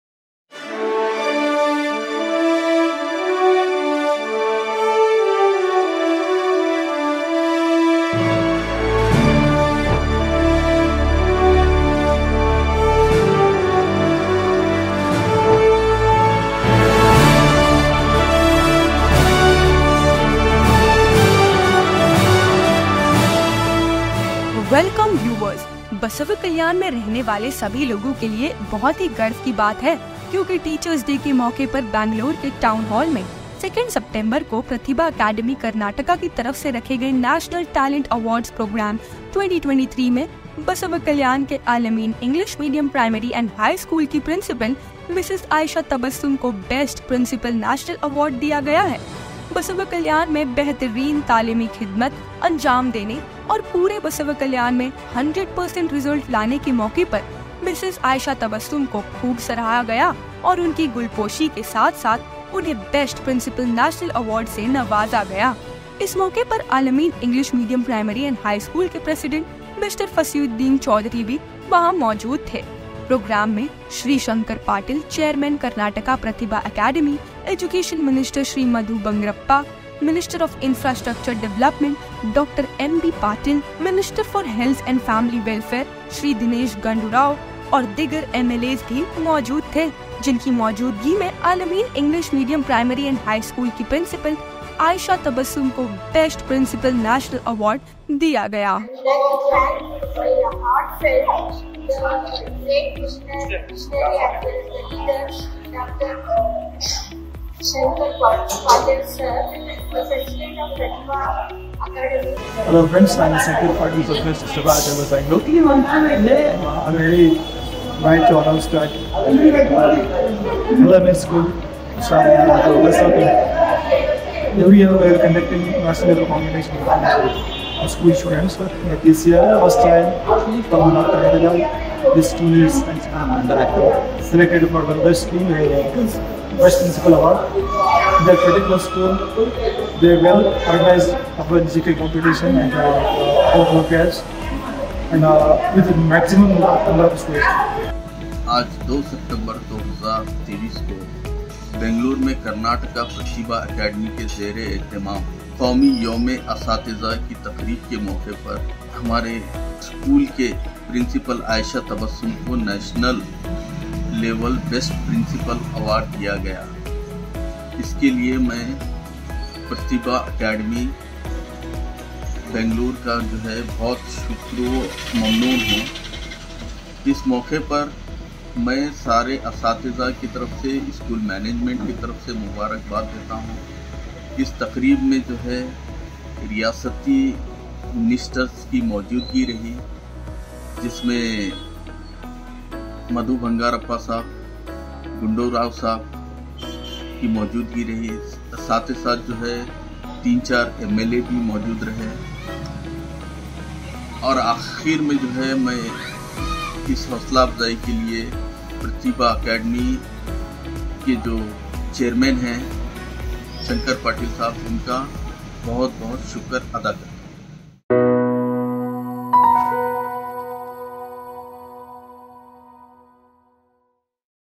Best Principal National Award | Ms. Aayesha Tabassum | Al-ameen English Medium Primary & High School
Al ameen English medium primary and High school Basavakalyan